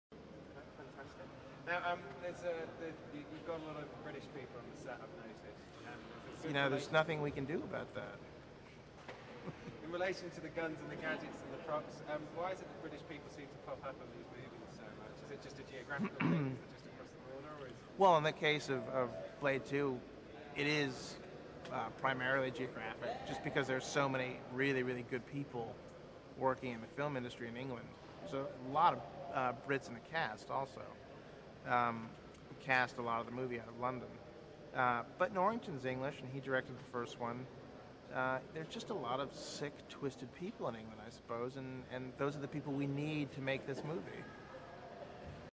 Gabriel Beristain on British Involvement in Blade II, 2000's - Film 93051
Interview with Gabriel Beristain, the Director of Photography (Cinematographer) of 'Blade 2' (2002). He is asked about why there are so many British people working onset in the Blade sequel.